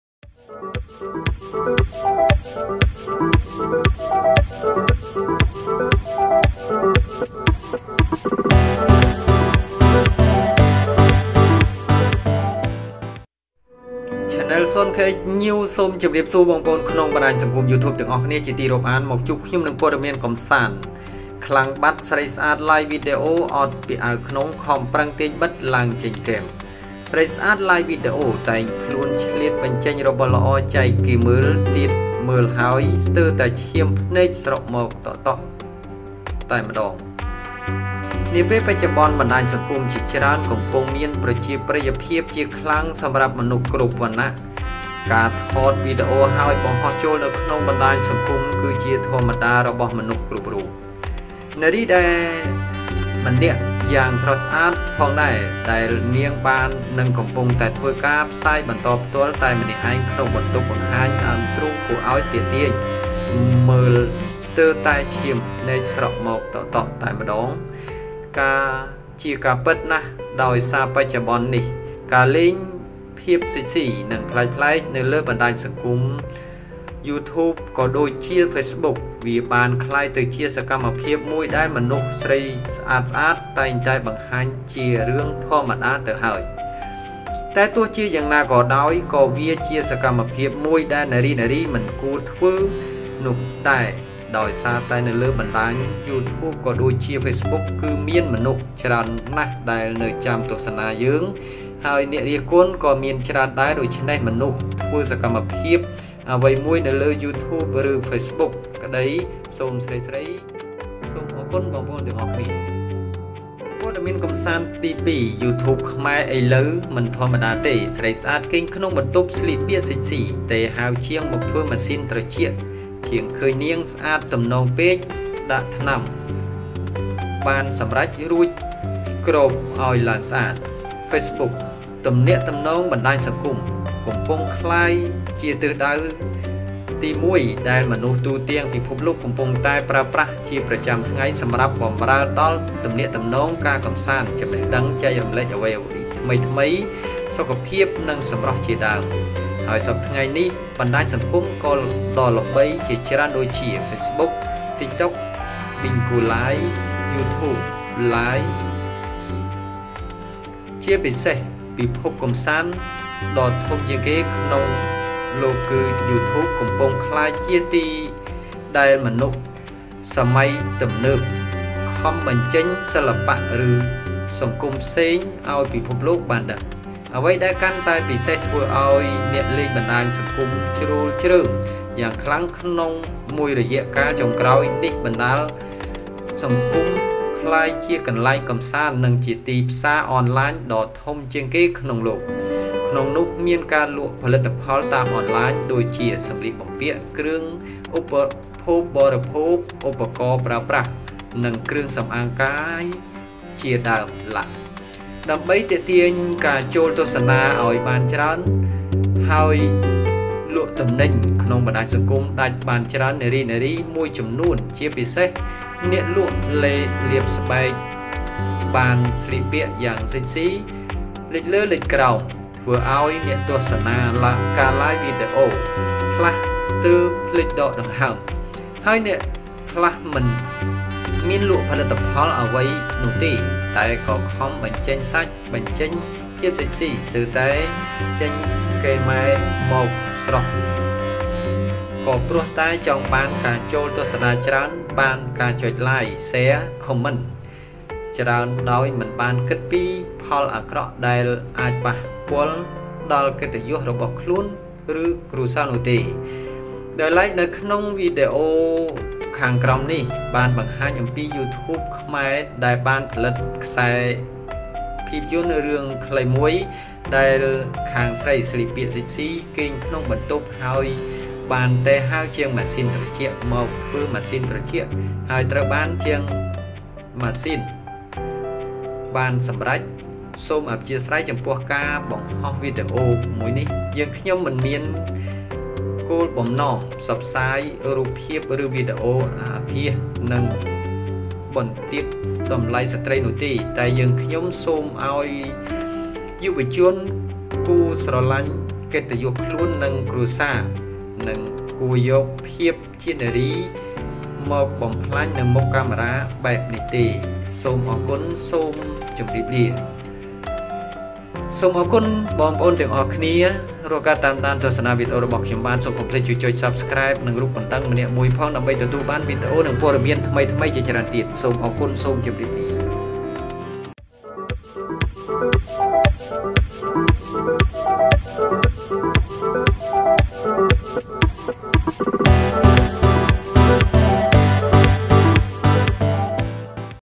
ខ្លាំងបាត់! ស្រីស្អាតLiveវីដេអូអត់ពាក់អាវក្នុង​ ខំប្រឹងទាញបិទឡេីងចេញគែម Yan Sun
ខ្លាំងបាត់! ស្រីស្អាតLiveវីដេអូអត់ពាក់អាវក្នុង​ ខំប្រឹងទាញបិទឡេីងចេញគែម

ព័ត៌មានថ្មីៗ
ព័ត៌មានទាន់ហេតុការណ៍ប្រចាំថ្ងៃ
ព័ត៌មានប្លែកៗនិងកំសាន្ត

បញ្ជាក់
ខ្លឹមសារវីដេអូ ក្នុង channel Yan Sun
មិនទាក់ទងក្នុងរឿងនយោបាយជាដាច់ខាត។